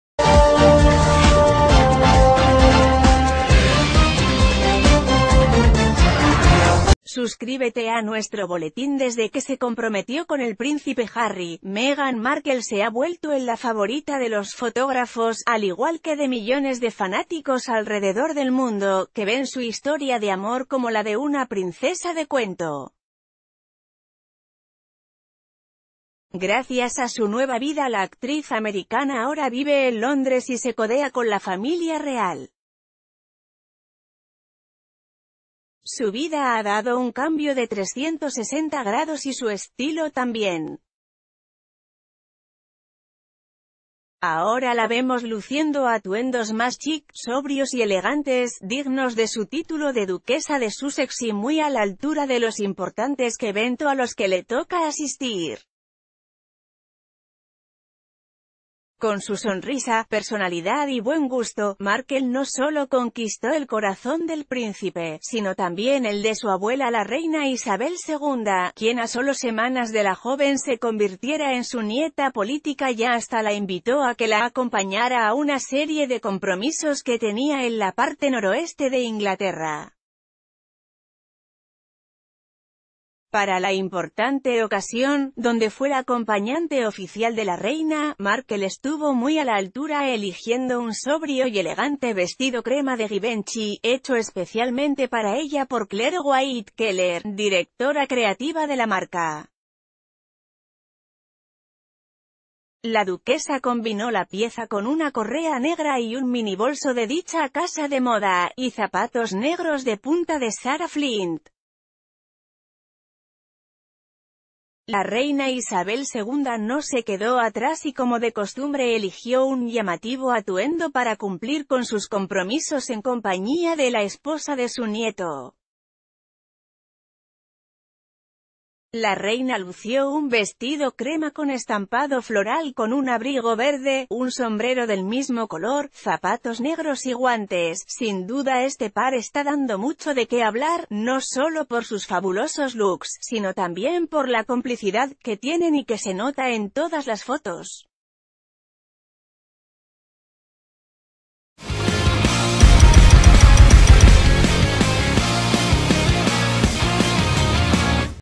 ¿Qué te parecen los looks de Meghan Markle y la reina Isabel en su primera salida juntas?
SUSCRÍBETE A NUESTRO BOLETÍN Desde que se comprometió con el príncipe Harry, Meghan Markle se ha vuelto en la favorita de los fotógrafos, al igual que de millones de fanáticos alrededor del mundo, que ven su historia de amor como la de una princesa de cuento. Gracias a su nueva vida la actriz americ...